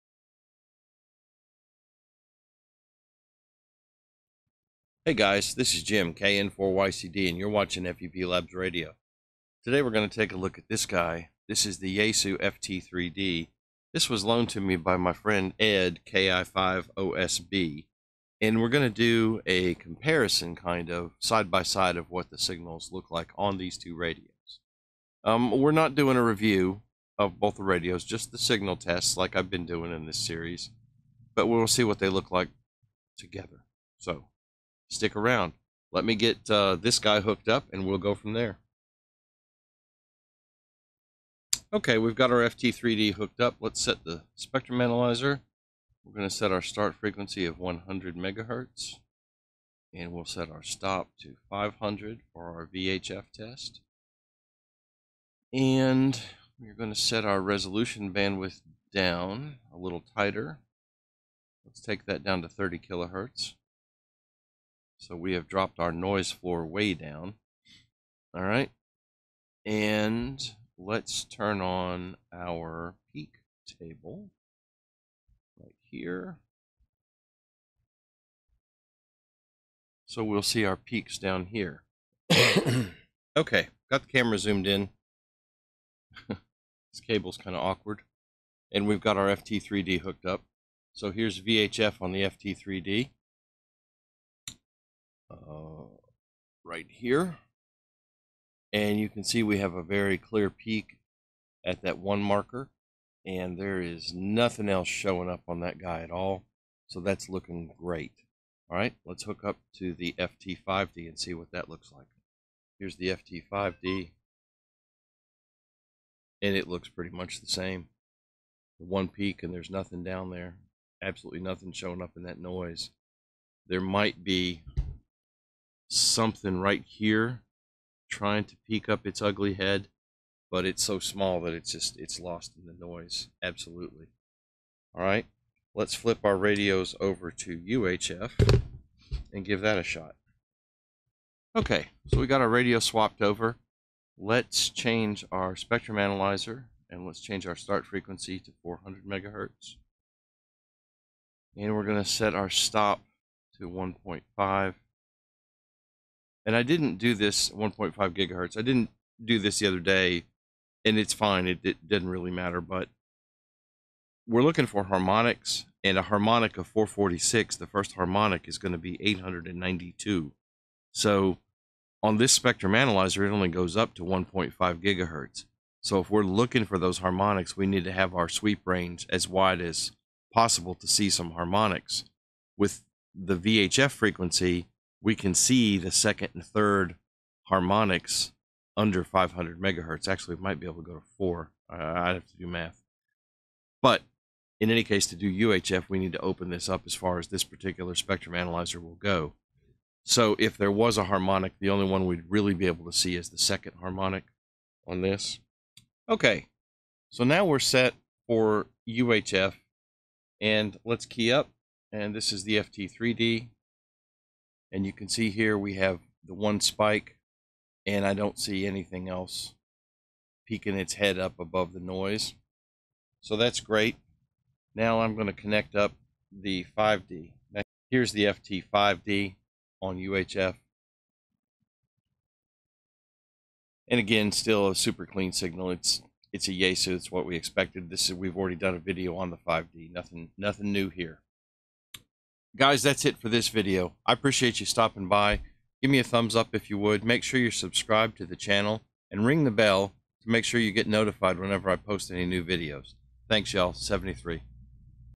Yaesu FT-3D vs FT-5D - Signal Check Series
The last (for a while) video in the series of signal tests!  I have the Rigol spectrum analyzer out and we do a comparison of the FT-3D vs the FT5D - how do they stack up?  Is the 5d more better awesomer??